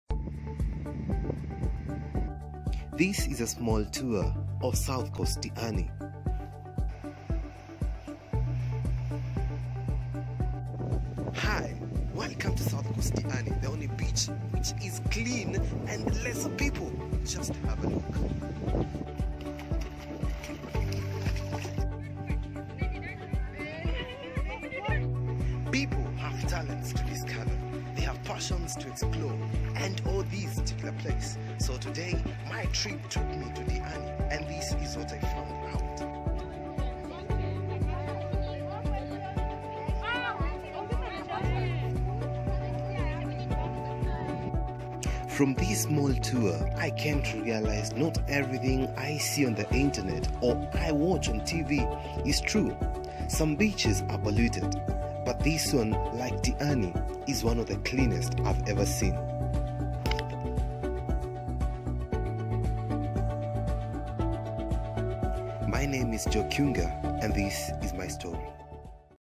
The best beach in Kenya. South Coast Diani
This is My Story 1st story about one of the Cleanest and well kept beach in Kenya. It's called Diani South coast.
My name is Joe and this is My Story